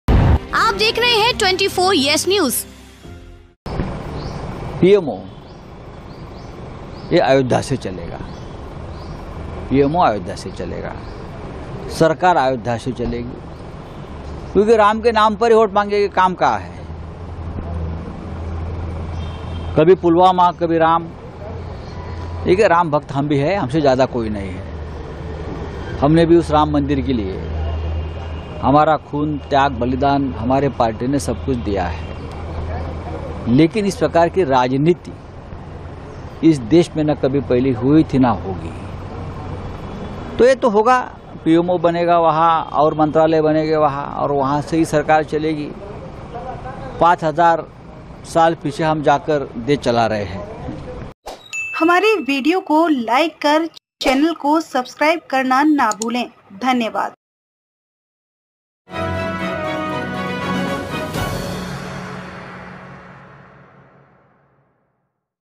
Government will function from Ayodhya. They will ask for votes only in Ram's name.
WATCH | On PM Modi's visit to Ayodhya ahead of the inauguration of Ram Temple, Shiv Sena (UBT faction) leader Sanjay Raut says, "...The PMO and government will function from Ayodhya. They will ask for votes only in Ram's name because they have done nothing else...Even we are devotees of Ram, in fact, we are the biggest devotees of Ram, and our party has sacrificed a lot for the Ram temple... They have taken our country back by 5000 years."